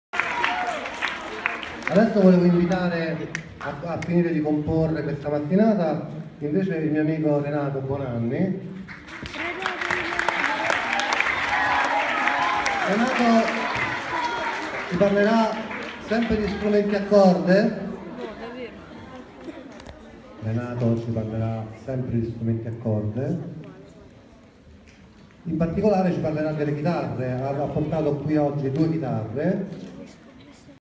Lezione concerto 1 - 06 Chitarra prima parte
Prima lezione concerto per gli alunni del IC Nelson Mandela, dedicata alle tastiere, gli archi e le chitarre.
Progetto a cura di Cesare del Prato con la partecipazione di Renato Bonanni, Quartetto Pessoa, Stefano Ferretti, Fabio Fochesato.
La lezione si è tenuta presso il Teatro Vascello il 10 Febbraio 2017.